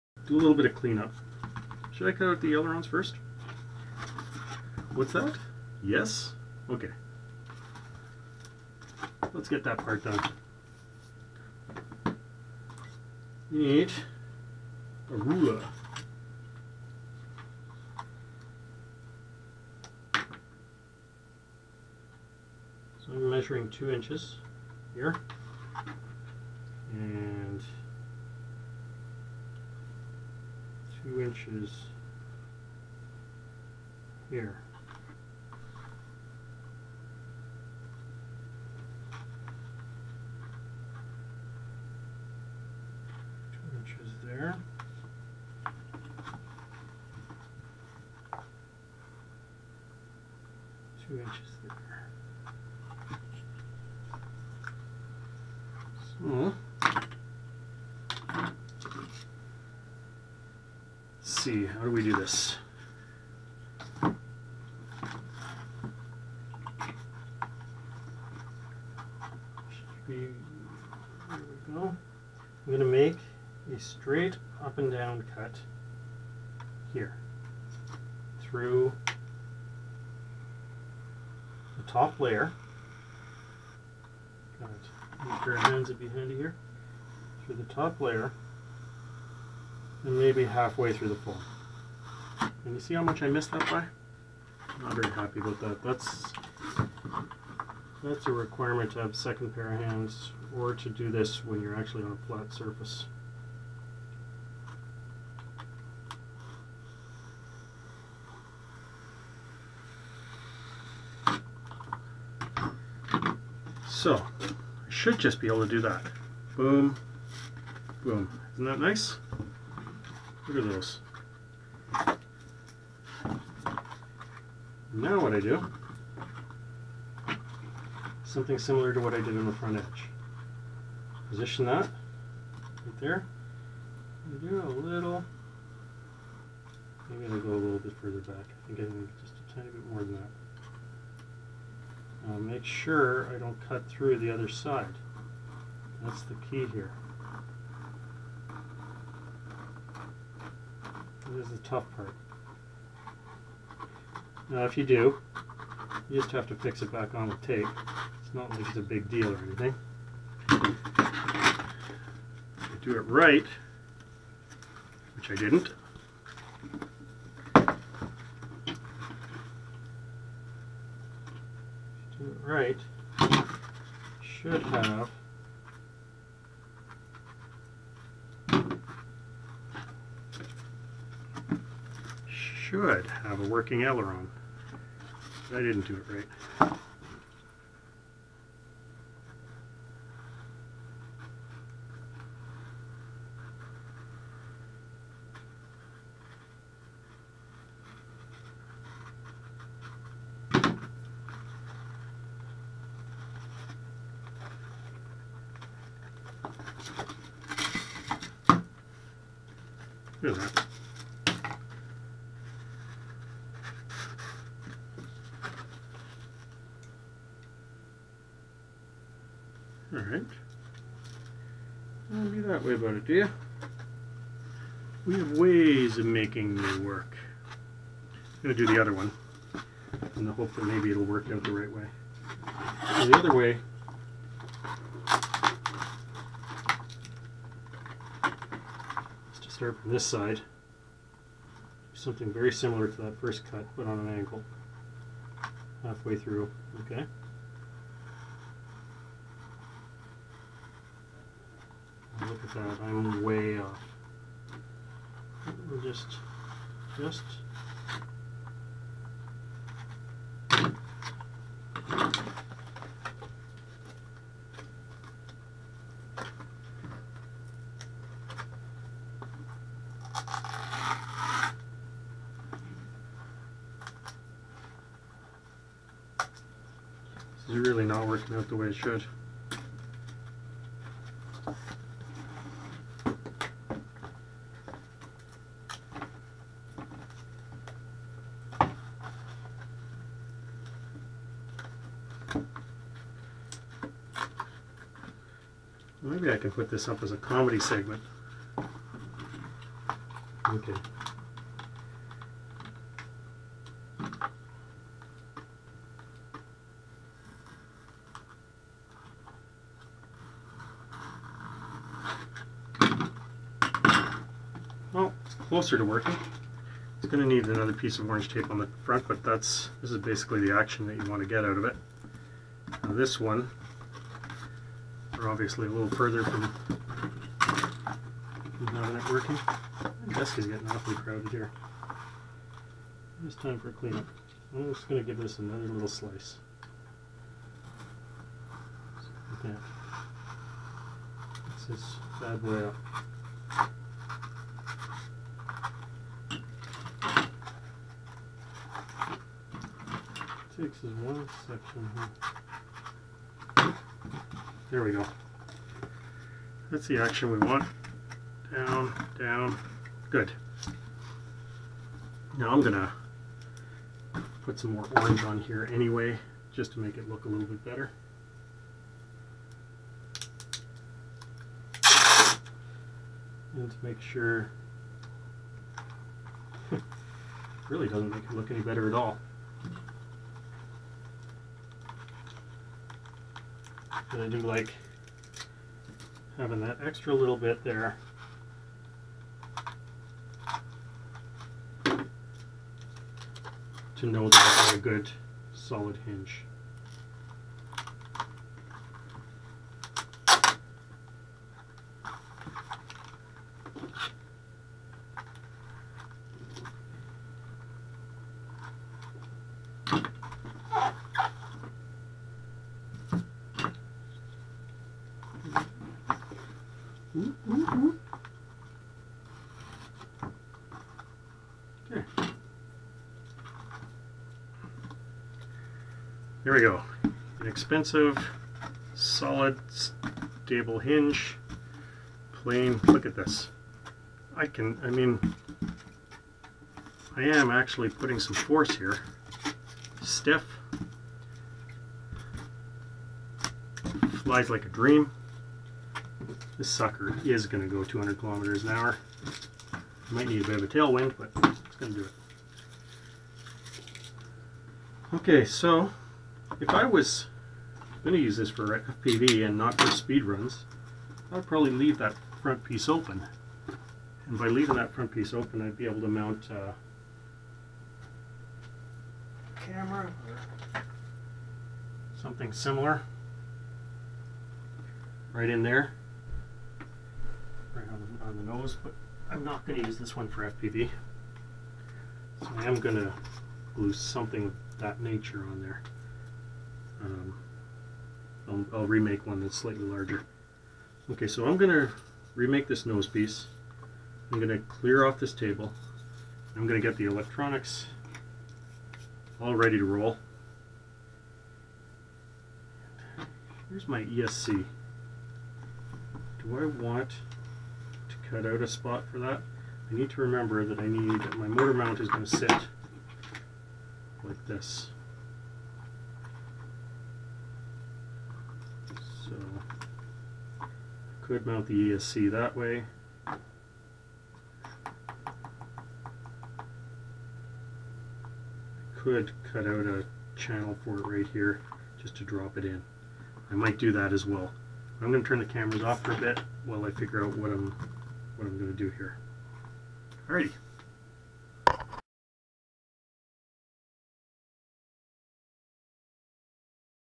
How to build a Flying Wing - Part 5 - Cutting the Elevons
Part 5 - Cutting the elevons

Fifth part of a multi part series on building the FPV30 wing.  This is a scratch build and will cost you between $35 and $60 depending on the electronics you choose.  The foam itself will cost less than $5.  Other videos in this series will include:

Installing the electronics
Final setup
Maiden flight